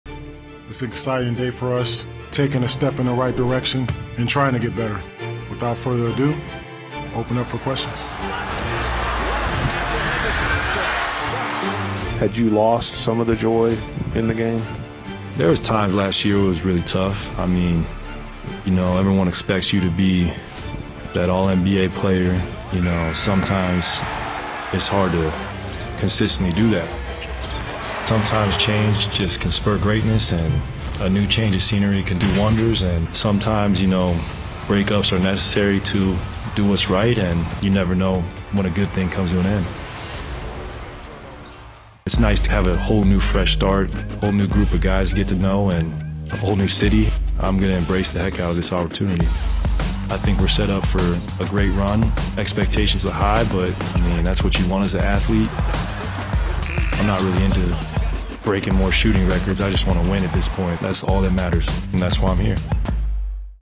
Klay Thompson on why he chose the Mavericks
Highlights from Klay Thompson's introductory press conference as a Dallas Maverick.

Watch your local teams! Visit ➡️ BallySports.com for more.